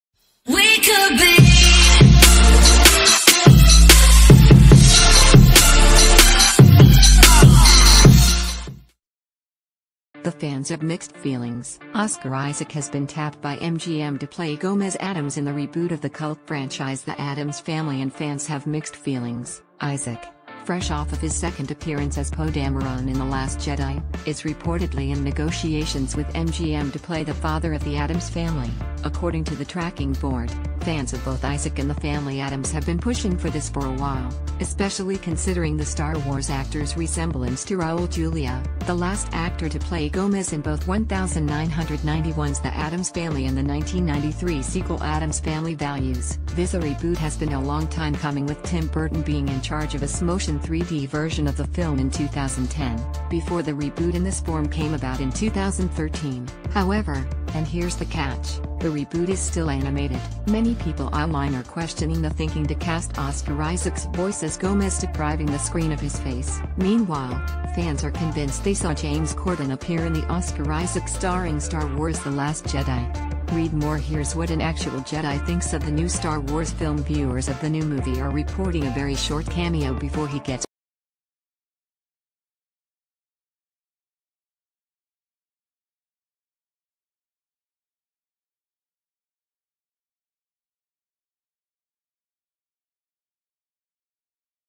‘addams family’ reboot in talks to cast oscar isaac as gomez addams
The fans have mixed feelings. Oscar Isaac has been tapped by MGM to play Gomez Addams in the reboot of the cult franchise ‘The Addams Family’ and fans have mixed feelings. Isaac, fresh off of his seco...